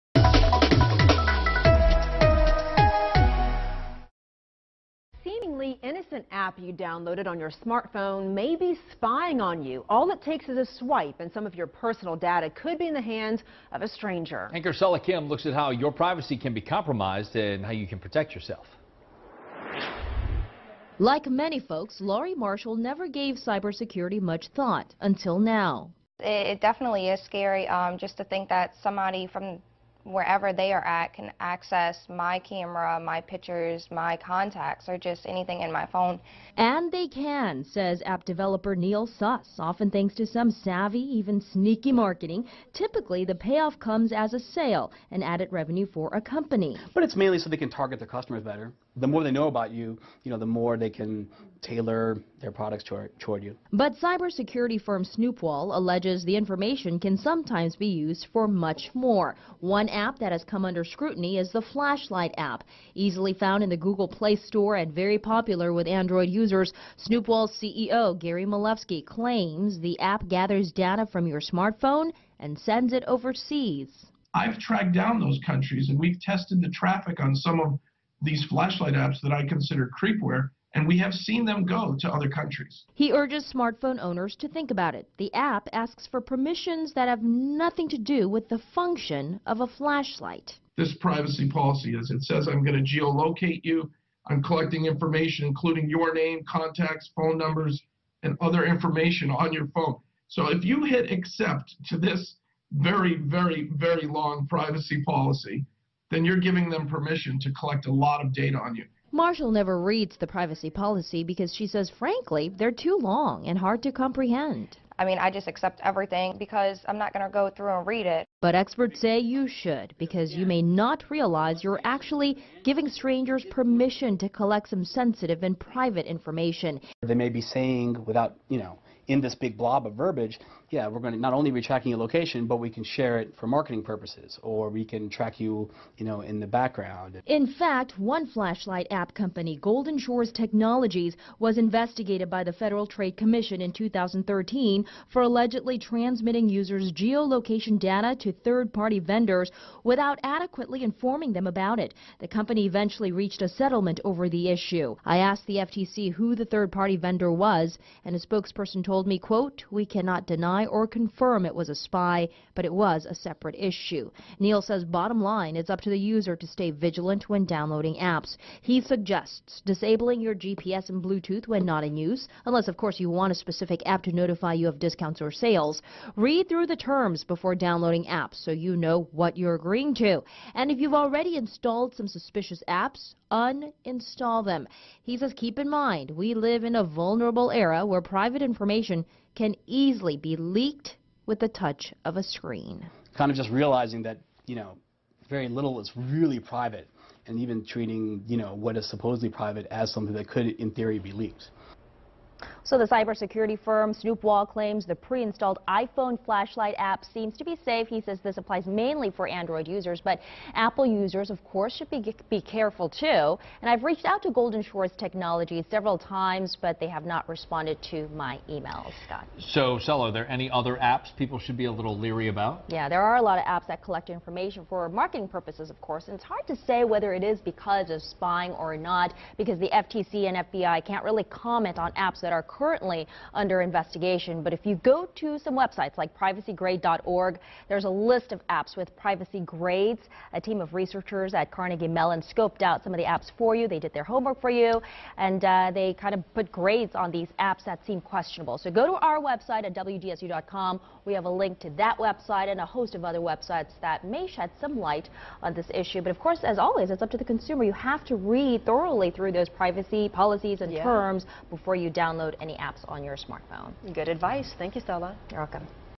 Your smart phone is Spying on You !
Your smart phone is Spying on You (Android-Google or Apple-iOS) ! Remove bad apps and personal accounts NOW !